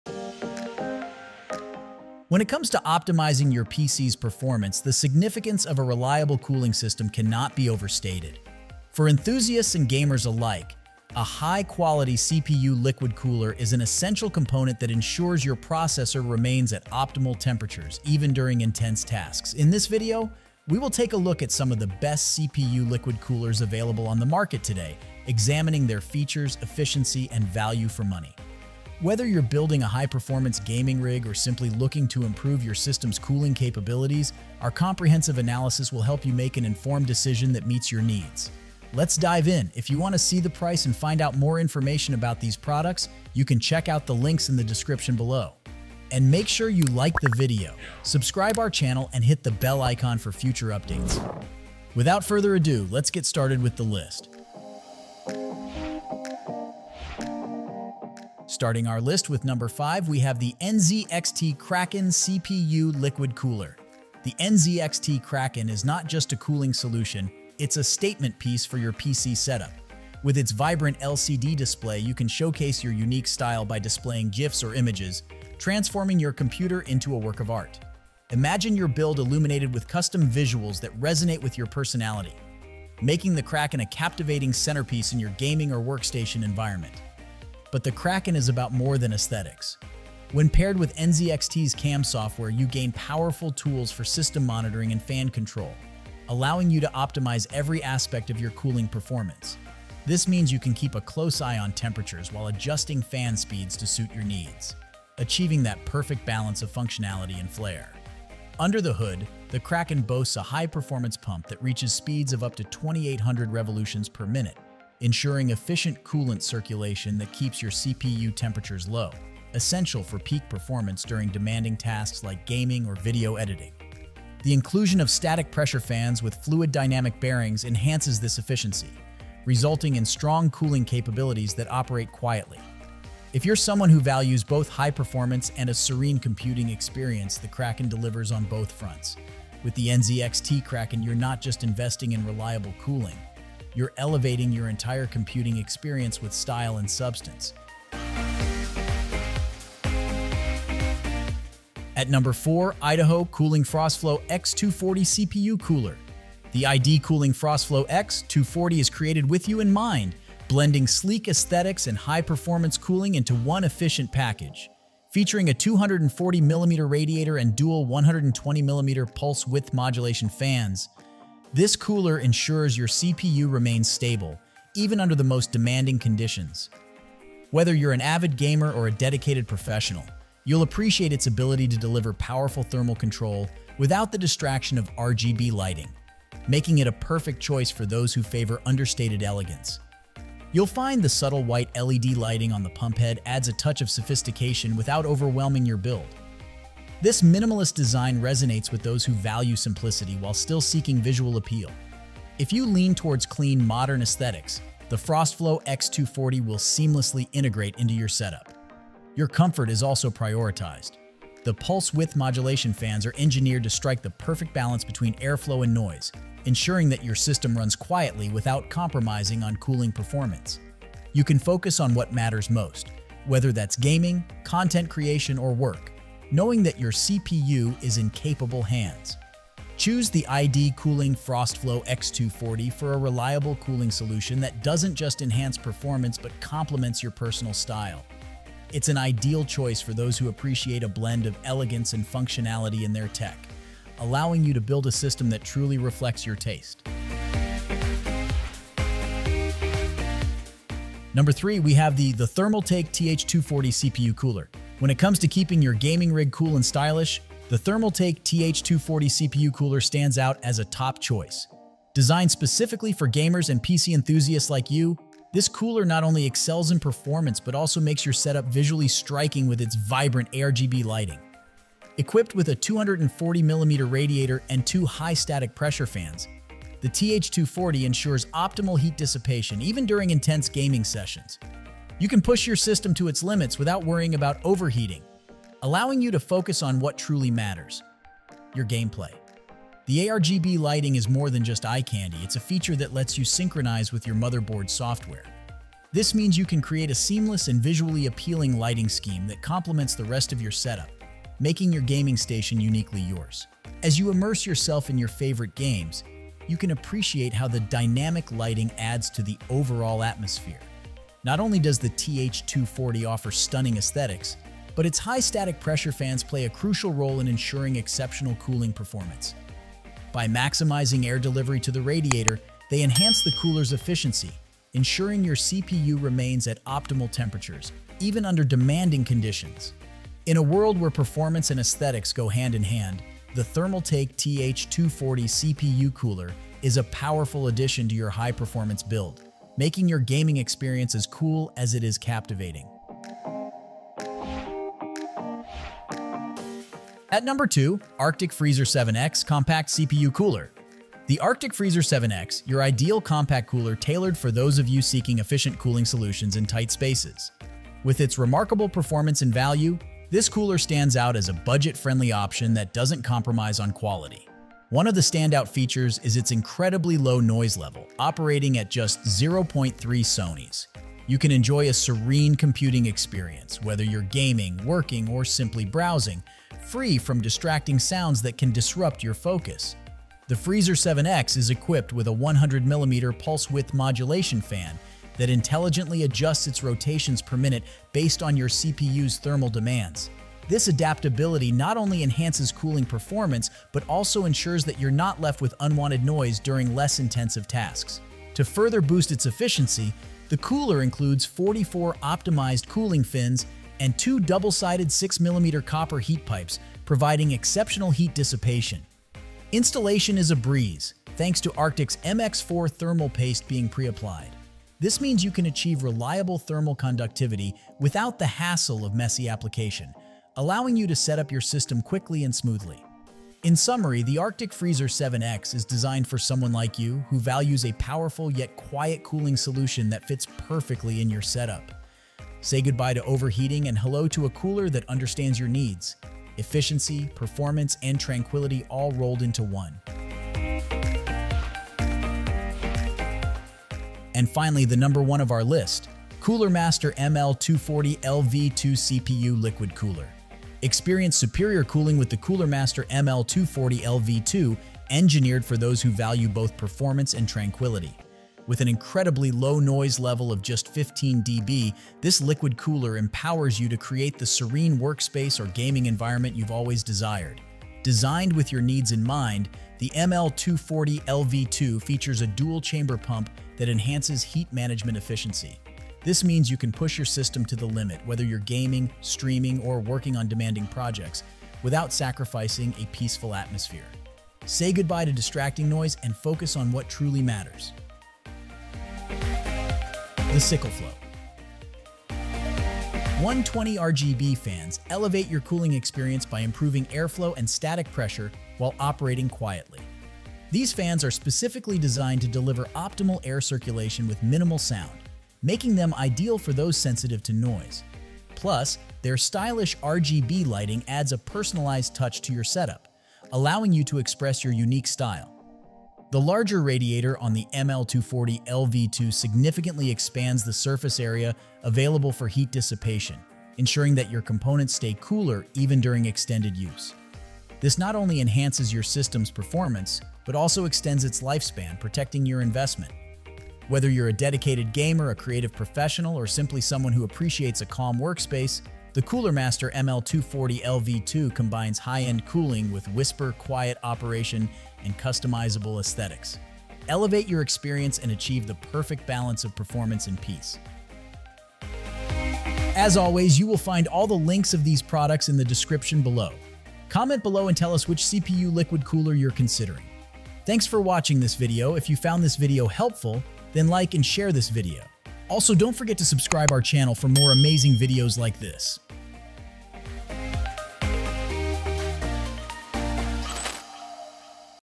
Best CPU Liquid Coolers 2025 - (Don’t Buy Without Watching This)
✅ List of the Best CPU Liquid Coolers we featured in this video

1. CoolerMaster ML240L V2 CPU Liquid Cooler

2. ARCTIC Freezer 7 X Compact CPU Cooler

3. the Thermaltake TH240 CPU cooler

4. ID-COOLING FROSTFLOW X 240 CPU Cooler

5. NZXT Kraken CPU Liquid Cooler

Keep your CPU running at peak performance with these top liquid coolers, designed to provide efficient and reliable cooling for your system. The CoolerMaster ML240L V2 CPU Liquid Cooler combines powerful performance with customizable RGB lighting, making it a stylish and efficient choice for gamers. The ARCTIC Freezer 7 X Compact CPU Cooler is perfect for smaller setups, offering excellent cooling in a space-saving design. For those who demand both power and aesthetics, the Thermaltake TH240 CPU Cooler delivers outstanding thermal performance with vibrant lighting effects. The ID-COOLING FROSTFLOW X 240 CPU Cooler is a budget-friendly option that doesn't compromise on performance. Finally, the NZXT Kraken CPU Liquid Cooler is a premium choice, offering exceptional cooling with a sleek design and intuitive software controls. Upgrade your PC cooling with these liquid coolers to ensure optimal performance and longevity.